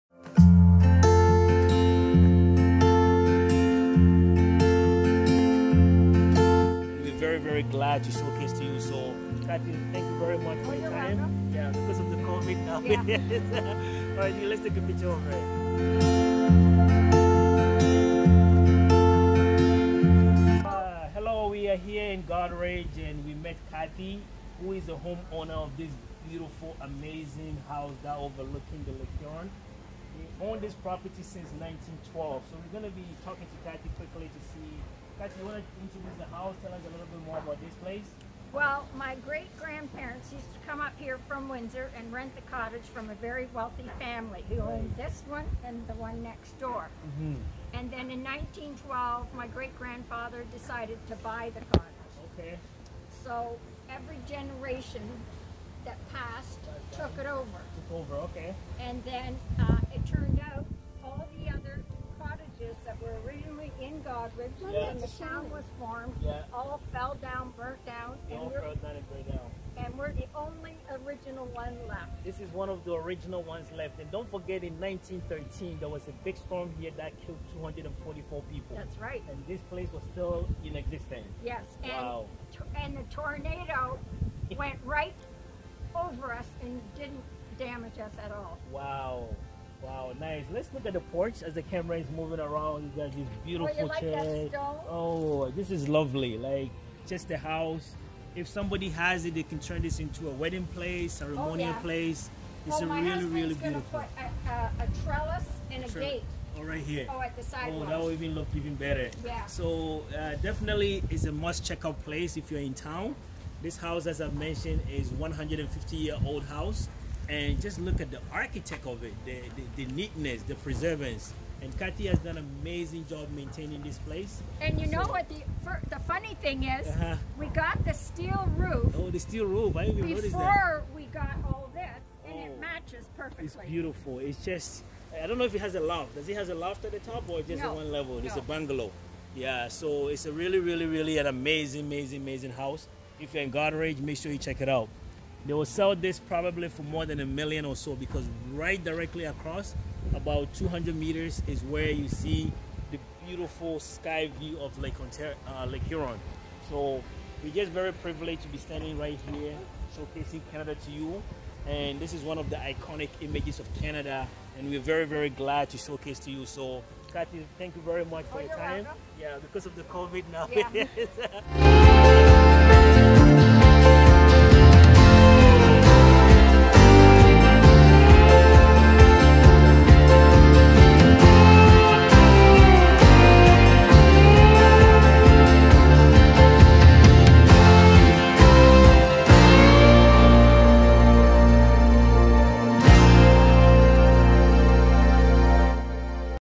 GODERICH, CANADA'S PRETTIEST TOWN | GODERICH HOUSE TOUR | GODERICH, ONTARIO
Goderich, Canada's prettiest town, Goderich house tour ,Goderich, Ontario is vlog showcasing a small town in Southern Ontario known for many attractions including Goderich cove beach, Goderich salt mine, lake huron, Menesetung Bridge, Goderich Lighthouse, Huron County Museum,  Huron Historic Gaol. In this vlog, however, we take a tour with Cathy on her 150 years old home inherited from her grandparents. This house is about 200 meters away from Lake Huron and the views are spectacular.

8. Director Of Photography (DOP): Kwabena Boateng